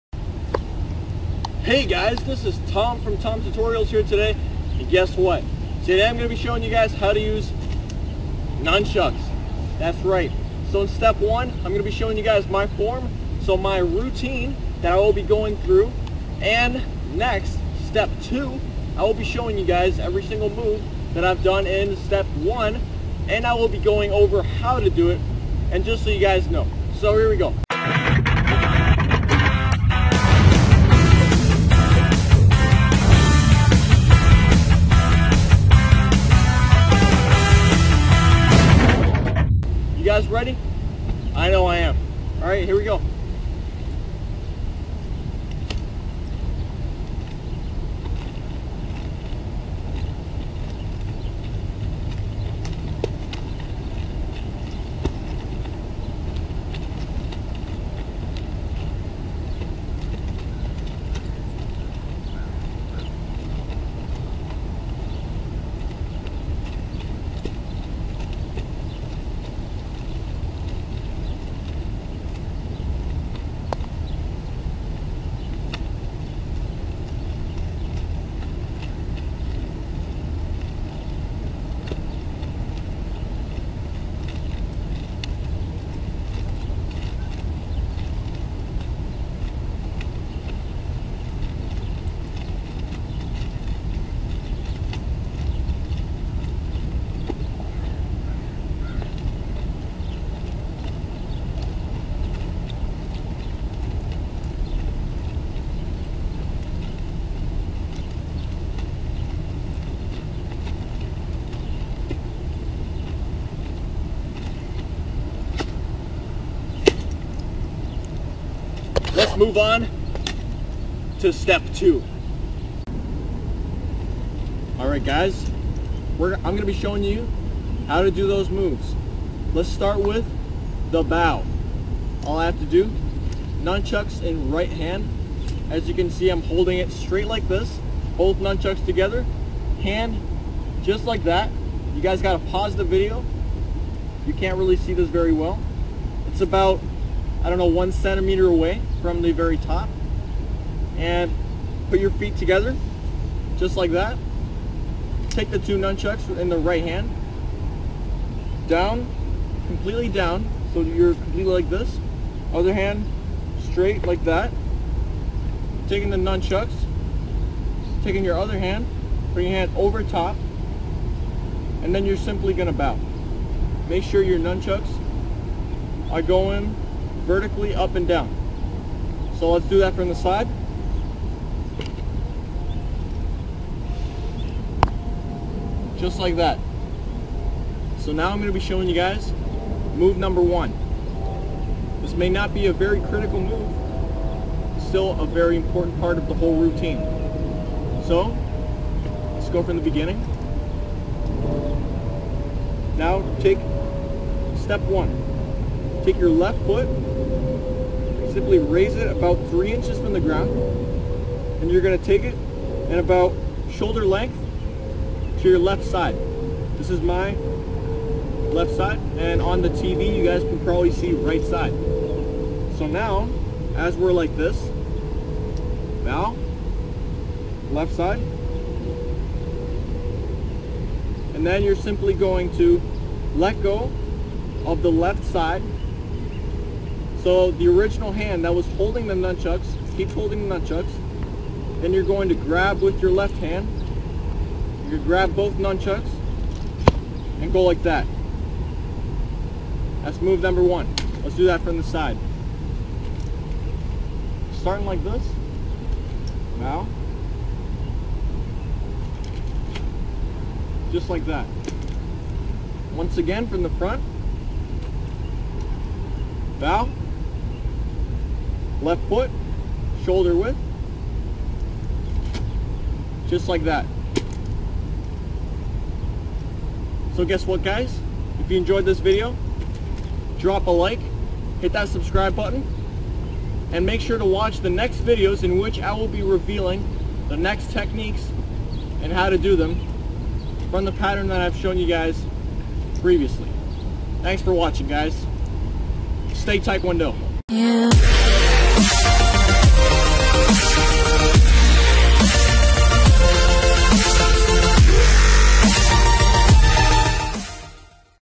How to Use Nunchucks: Episode 1 [TAE KWON DO]
This is the very first video in my Tae Kwon Do series on how to use nunchucks!